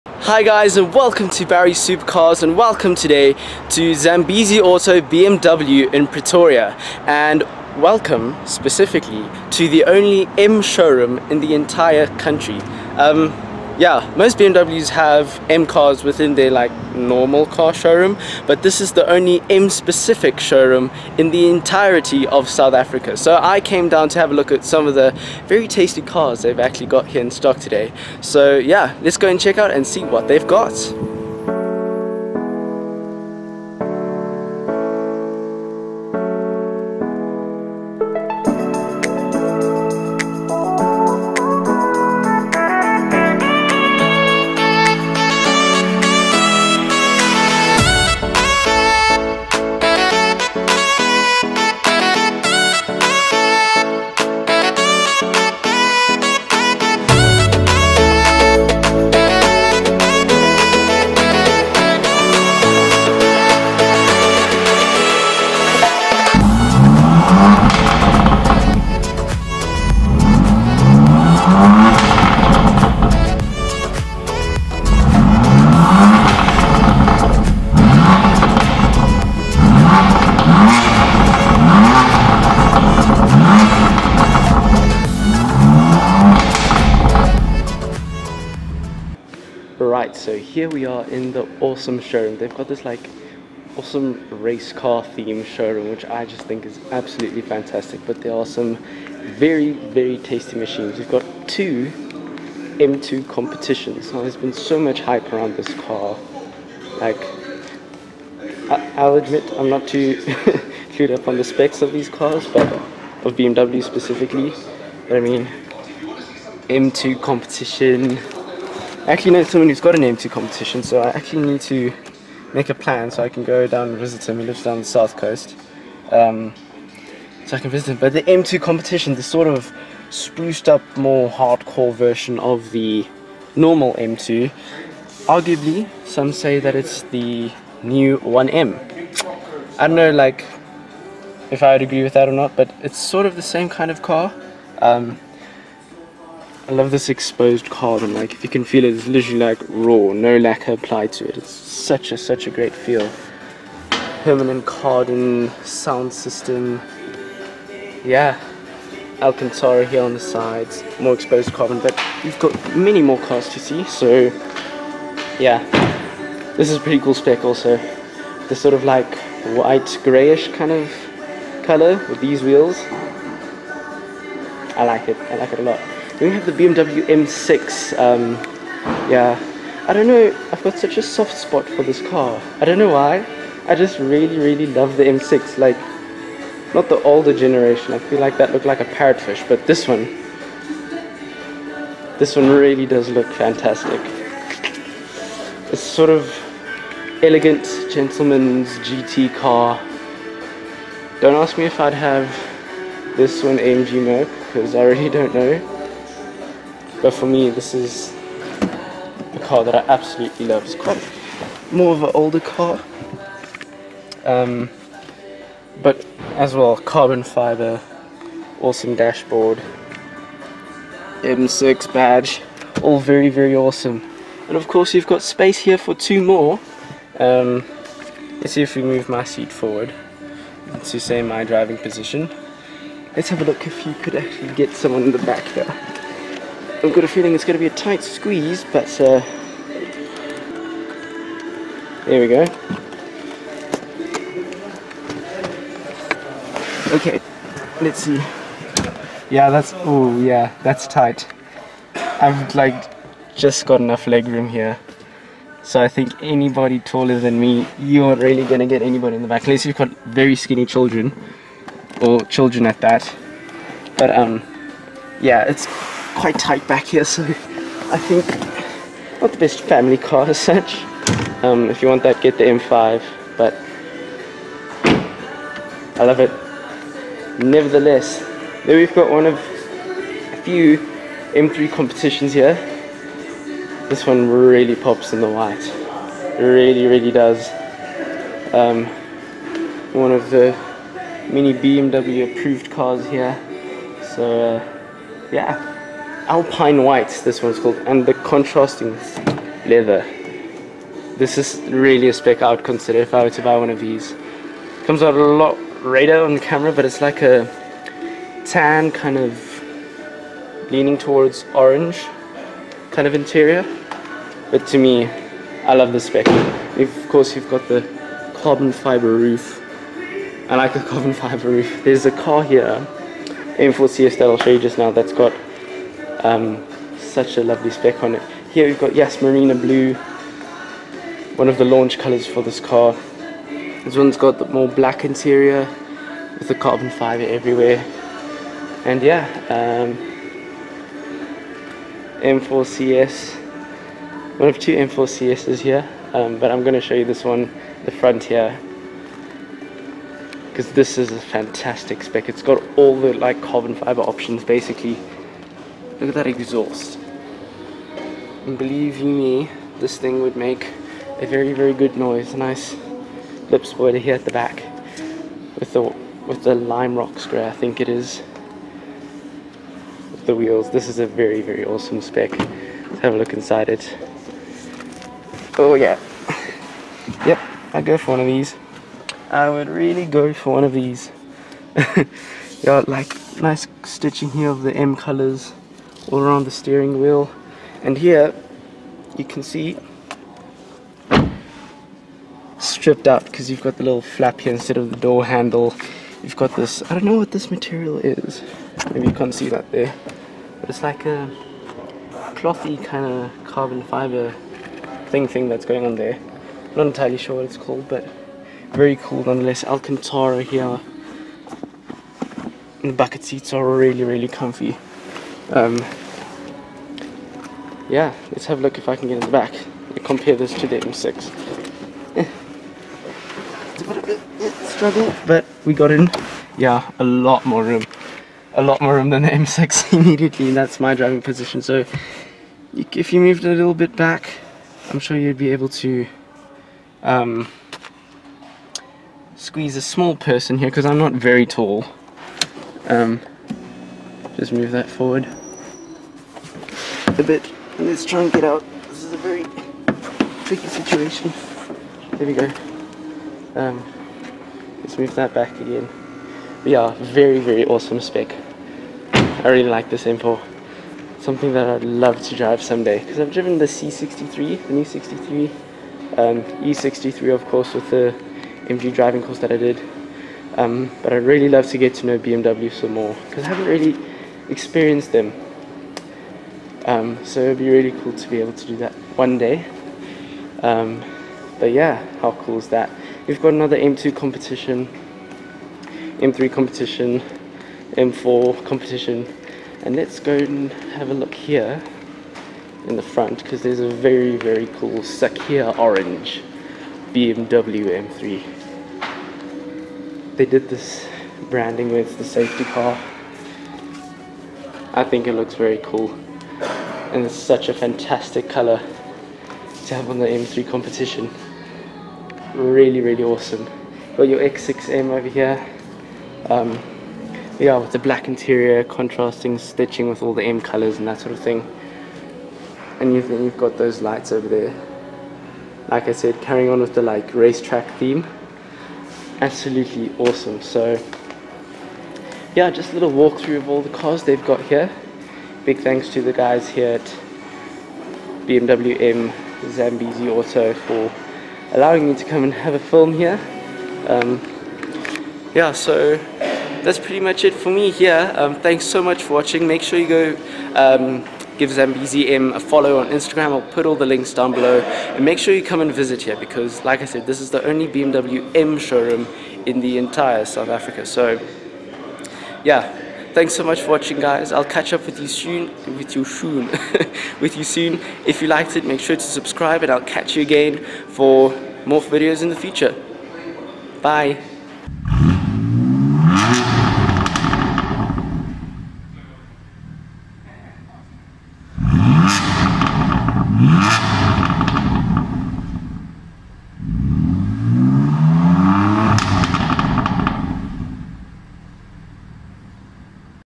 Visiting the ONLY BMW M showroom in South Africa! M2 Competition, M4 Revs
Hi guys!

Seeing I live nearby so many dealerships now, I decided to go and have a look at Zambesi Auto BMW's M Showroom. They had some pretty awesome cars on the floor! This was pretty much the first time I've been around BMWs and I'd definitely like to experience one at some point!